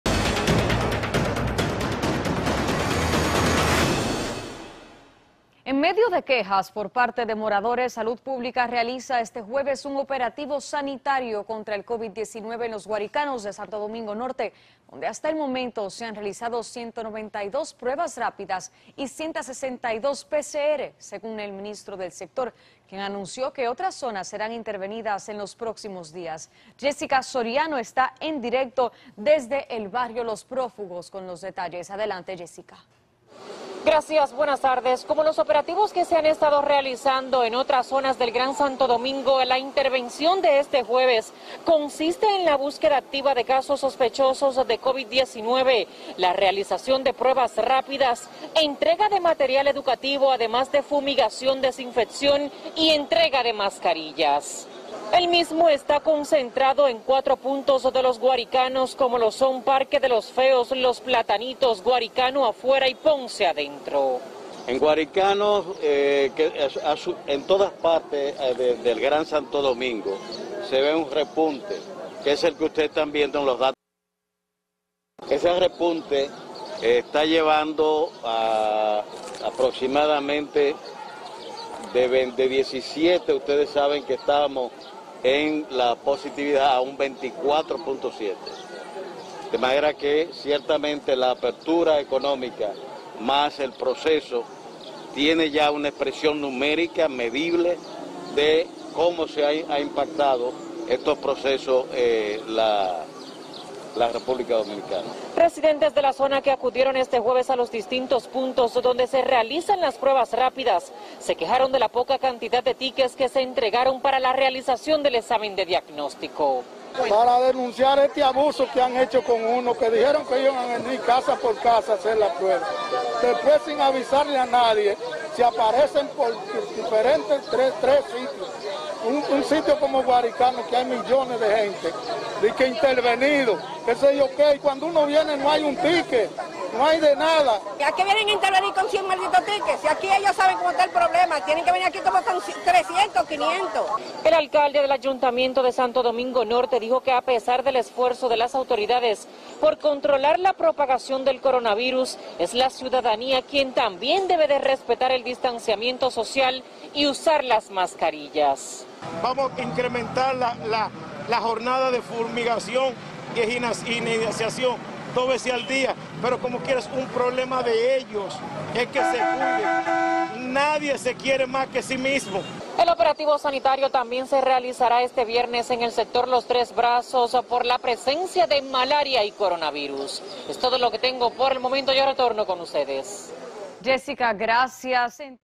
Salud Pública interviene Los Guaricanos
En medio de quejas por parte de moradores, Salud Pública realiza este jueves un operativo sanitario contra el COVID-19 en Los Guaricanos, de Santo Domingo Norte, donde hasta el momento se han realizado 192 pruebas rápidas y 162 PCR, según el ministro del sector, quien anunció que otras zonas serán intervenidas en los próximos días.